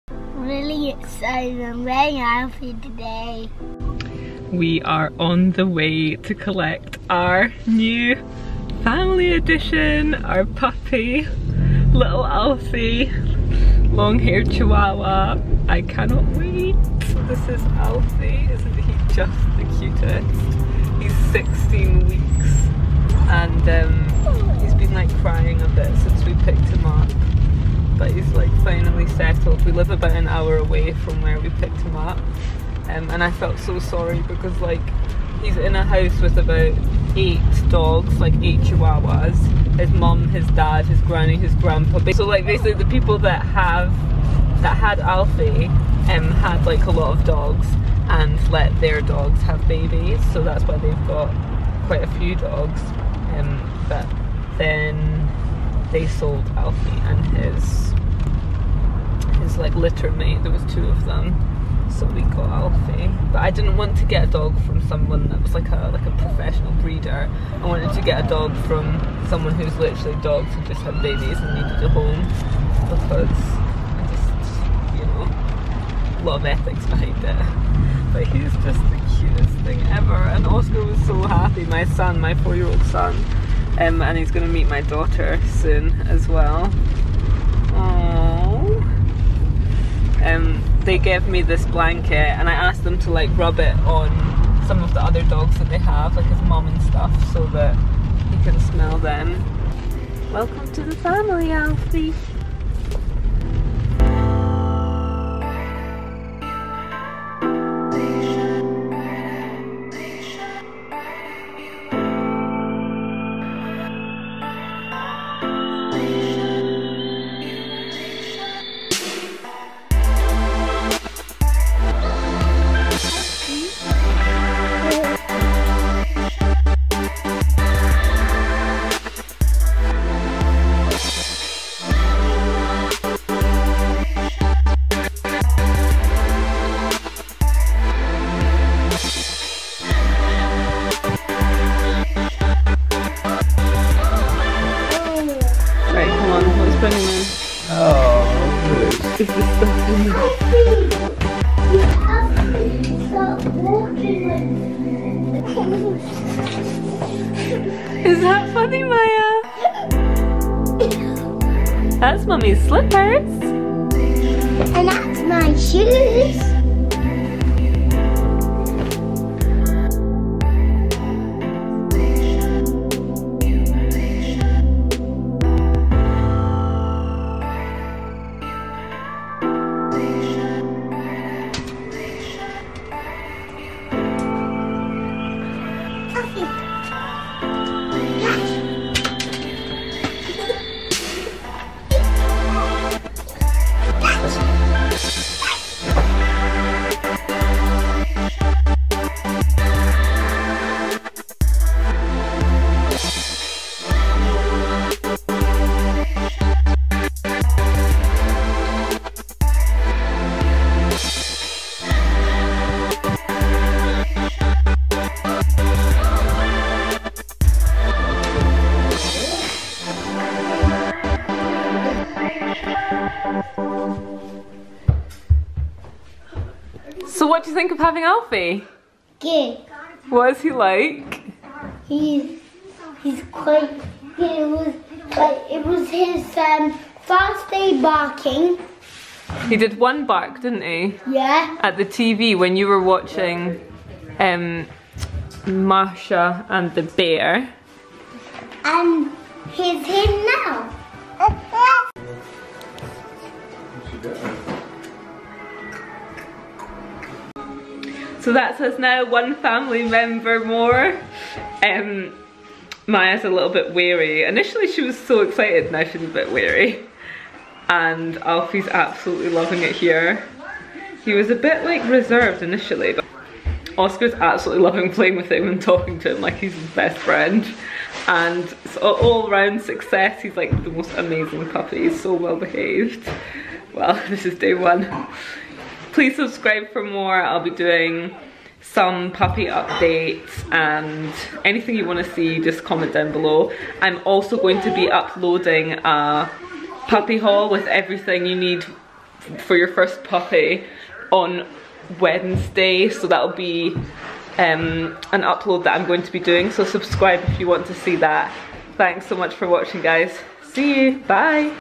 PUPPYS FIRST DAY HOME | Long Haired Chihuahua - New Family Dog
Today we collected our new puppy - a long haired chihuahua called Alfie! He is the most adorable dog in the world! We take you with us when we go and collect our new puppy & when we introduce him to my baby and son!